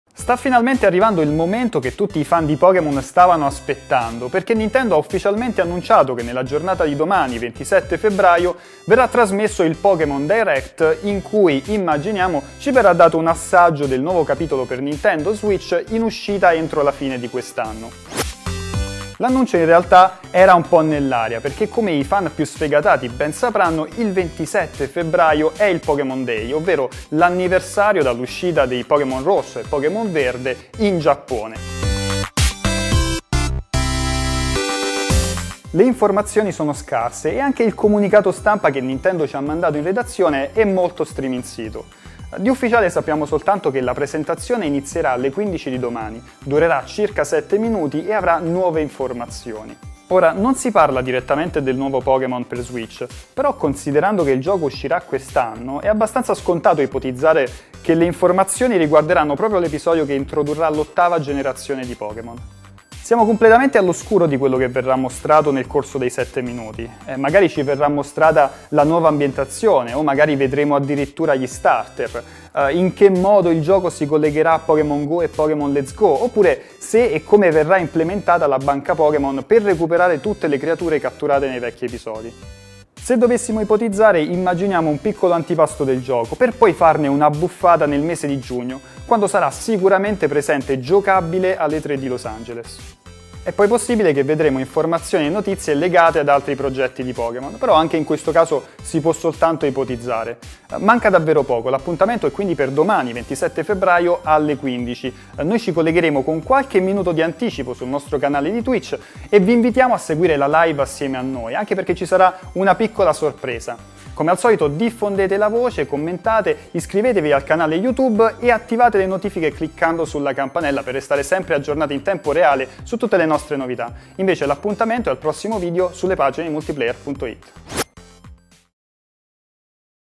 Pokémon Direct: novità in arrivo per Pokémon 2019 su Switch!
Nintendo ha ufficialmente annunciato che domani, 27 febbraio alle ore 15:00 italiane verrà trasmesso il Pokémon Direct in cui - possiamo starne certi - ci verrà dato un assaggio del nuovo capitolo in arrivo su Nintendo Switch. Nel video tutti i dettagli, mentre l'appuntamento è per domani sul nostro canale Twitch